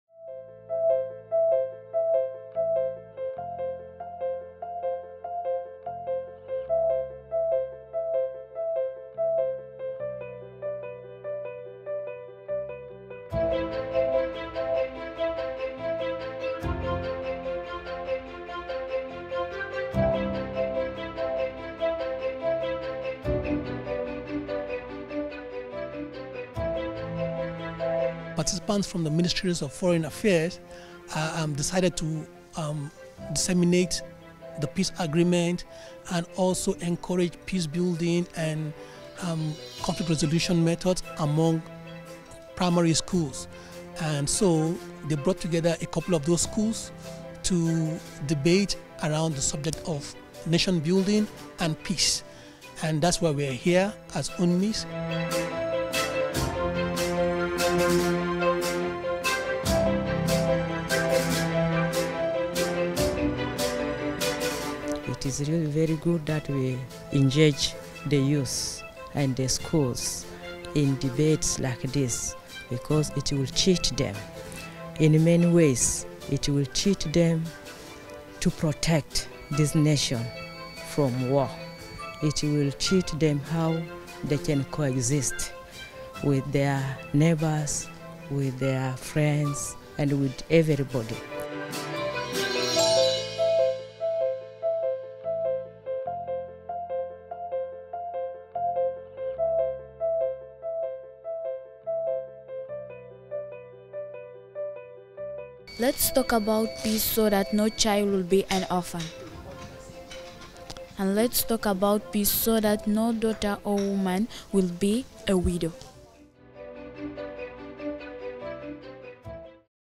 UNMISS teams up with South Sudan's Ministry of Foreign Affairs to hold student debate
As SouthSudan stands at a pivotal time in its democratic transition, UNMISS in partnership with the Ministry of Foreign Affairs holds an engaging and candid debate on nation building by secondary school students.

Video by Sonam Tharchen/UNMISS